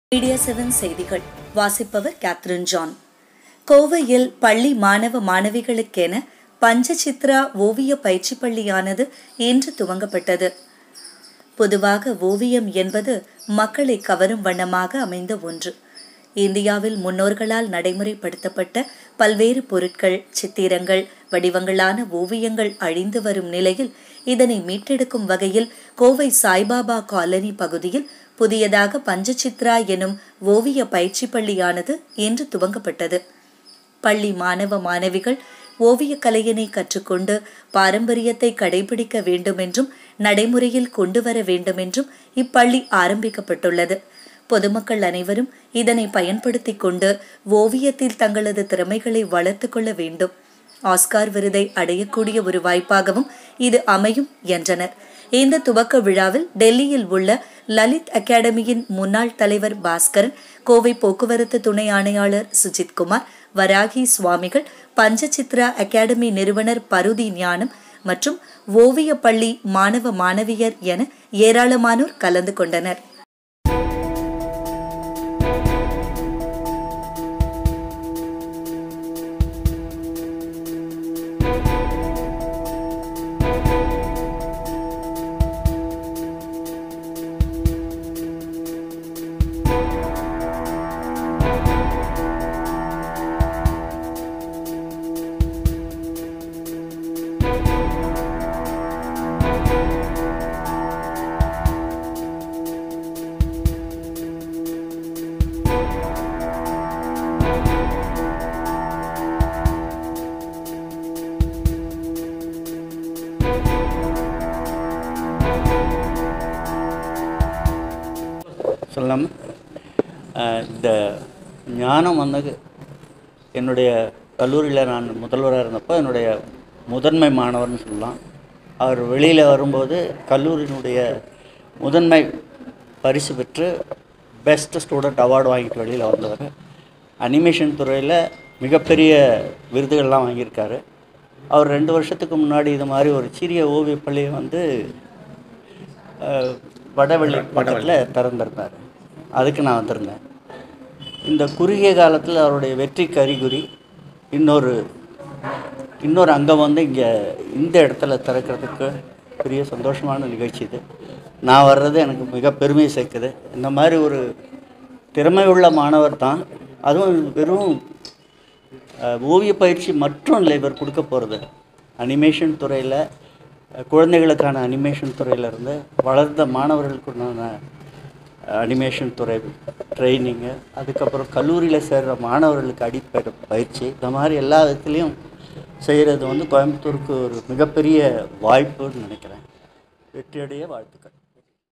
கோவையில் பஞ்ச சித்ரா ஓவிய பயிற்சி பள்ளி துவங்கப்பட்டது.
கோவையில் பஞ்ச சித்ரா ஓவிய பயிற்சி பள்ளி
துவங்கப்பட்டது.

கோவையில் பள்ளி மாணவ மாணவிகளுக்கென பஞ்ச சித்ரா ஓவிய பயிற்சி பள்ளியானது இன்று துவங்கப்பட்டது.

      பொதுவாக ஒவியம் என்பது மக்களிடையே கவரும் வண்ணமாக அமைந்து வருகிறது. இதை தொடர்ந்து   இந்தியாவில் முன்னோர்கள் நடைமுறைப்படுத்தப்பட்ட பல்வேறு பொருட்கள் மற்றும் சித்திரங்கள்,வடிவங்களான ஓவியங்கள் அழிந்து வரும் நிலையில் இதனை மீட்டெடுக்கும் வகையில் கோவை சாயிபாபா காலனி பகுதியில் புதியதாக பஞ்ச சித்ரா எனும் ஓவிய பயிற்சிப்பள்ளியானது இன்று துவங்கப்பட்டது.இதில் பள்ளி மாணவ மாணவிகள் இந்த ஓவிய கலையினை கற்றுக்கொண்டு பாரம்பரியத்தை கடைபிடிக்க வேண்டுமென்றும் நடைமுறையில் கொண்டு வரவேண்டுமெண்டும் ஆரம்பிக்கப்பட்டது. இதன் மூலம் பொதுமக்கள் அனைவரும் பயன் படுத்தி கொண்டு தங்களது திறமைகளை ஒவியத்தின் மூலம் ஆஸ்கார் விருதை அடையக் கூடிய ஒரு வாய்ப்பாக இருக்கும் என்றனர். இந்த துவக்க விழாவில் டெல்லியில் உள்ள லலித் அகடாமியின் முன்னாள் தலைவர் பாஸ்கரன் ,மற்றும் கோவை போக்குவரத்து துணை ஆணையாளர் சுஜித்குமார், வராகி சுவாமிகள்,  பஞ்ச சித்ரா  அகடாமி நிறுவனர்  பருதி ஞானம் , மற்றும்  ஓவிய பள்ளி மாணவ மாணவியர் என ஏரளமனோர் கலந்துகொண்டனர்.